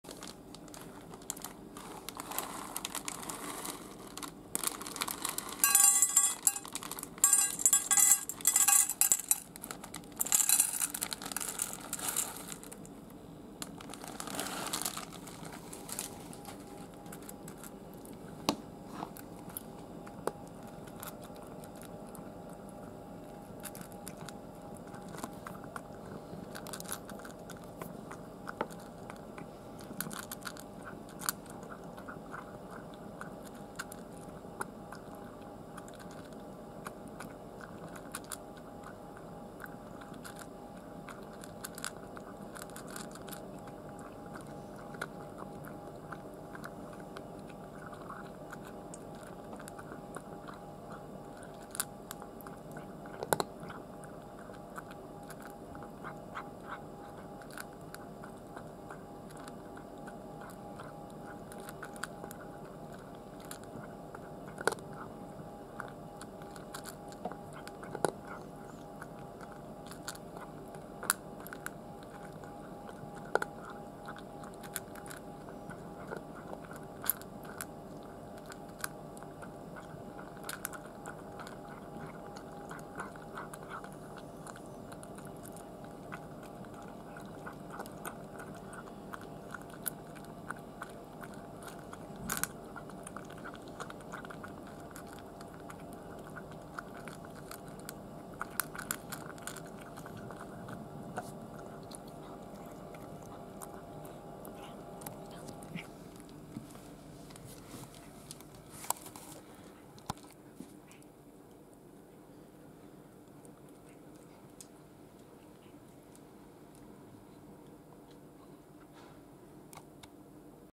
朝ごはんを食べる猫のゆいちゃん 2016/6/28
年齢は22歳です（＾－＾）
うちで飼っている猫です（＾－＾）ｂ
朝ごはんを食べていますΣ(･ω･ﾉ)ﾉ！
まだまだ元気いっぱいのネコちゃんです♪